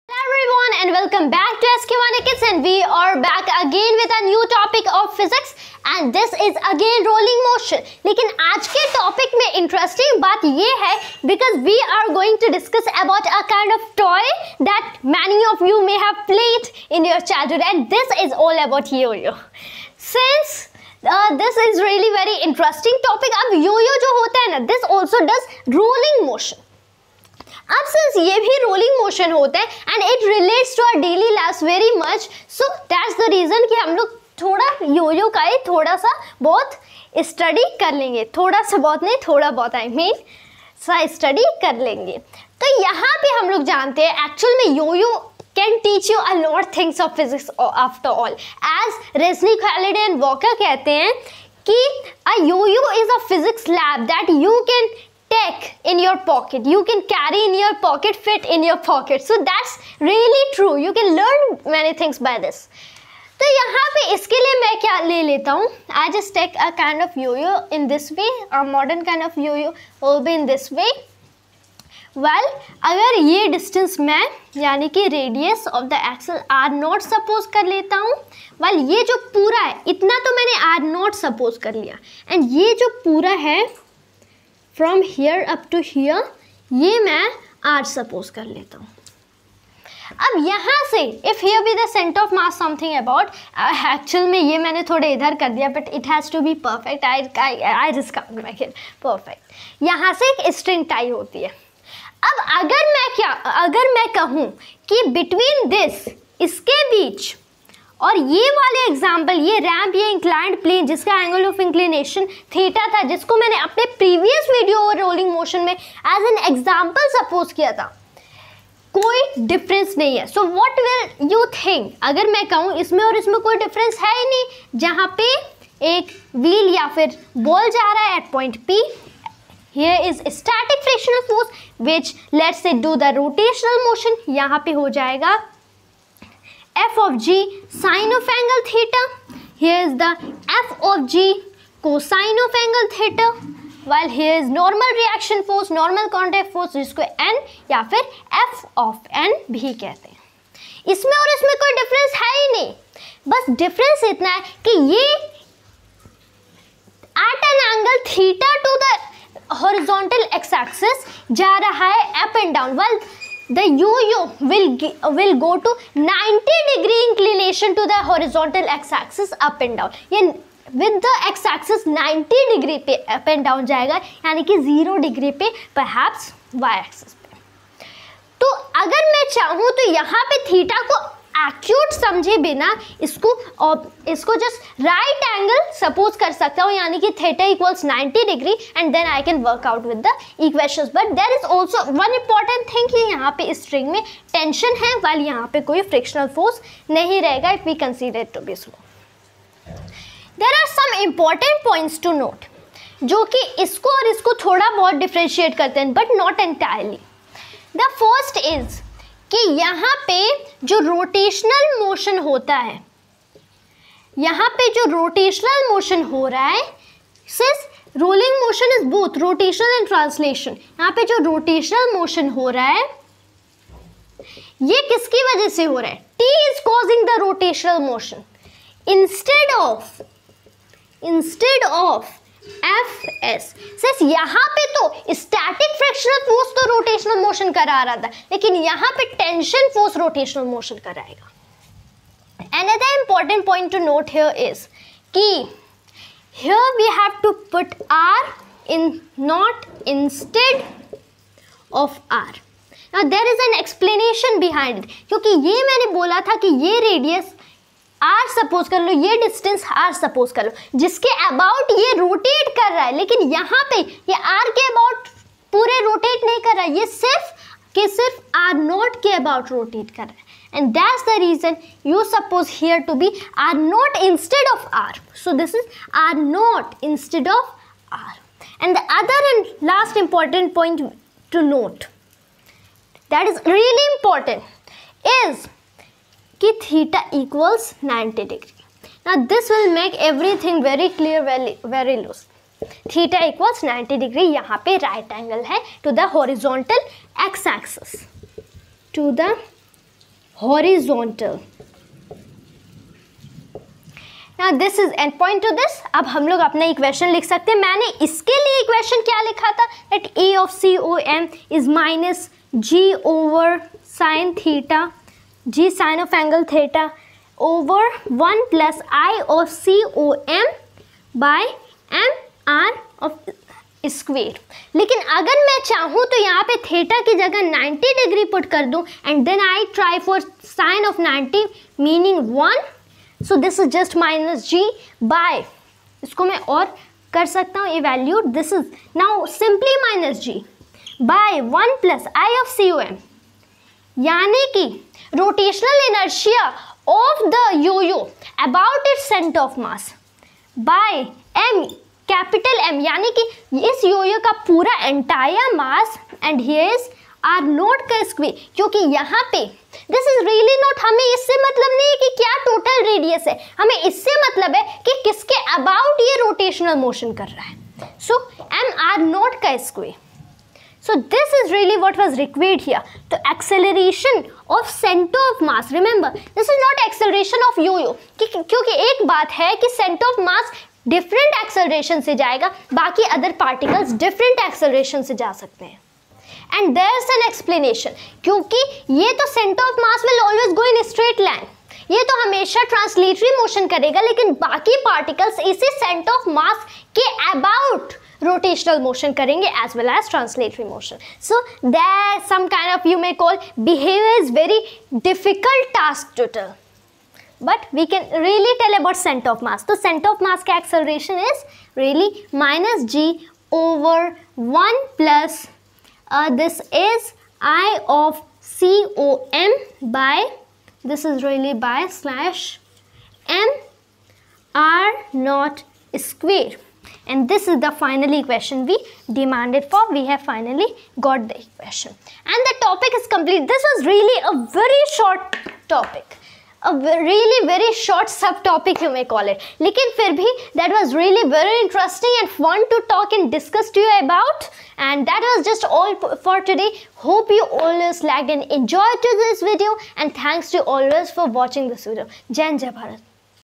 Yo-Yo|Rolling Motion|JEE|NEET|Class 11|Sarim Khan
Hello everyone
Today we are going to study about the physics behind a yo yo with Sarin Khan.